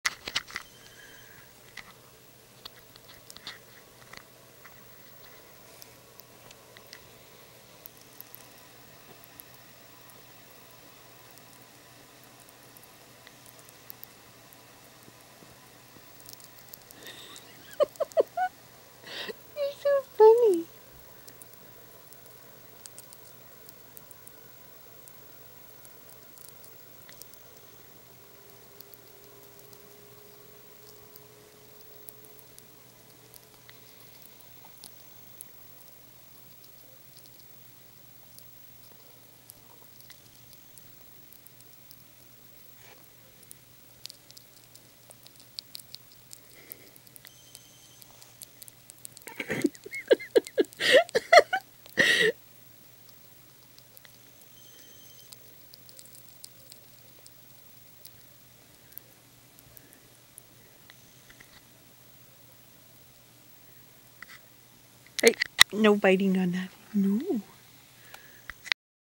My cute funny teddy bear hamster eating banana
Buster the hamster loves banana and he eats it very loudly.